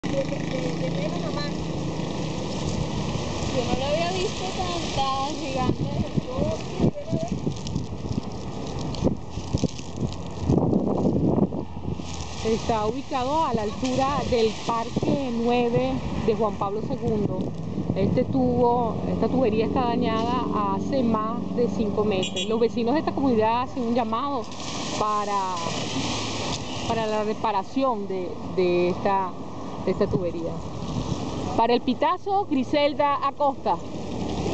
Piden reparación de huecos en avenida Teherán de Montalbán
A la altura del parque nueve de la Urbanización Juan Pablo II de Montalbán se encuentran dos huecos llenos de agua debido a la rotura de una tubería. Habitantes de Montalbán hace un llamado a la cuadrilla de mantenimiento de Hidrocapital para que realicen esta reparación ubicada en la avenida Teherán.  Temen por accidentes en horas de la noche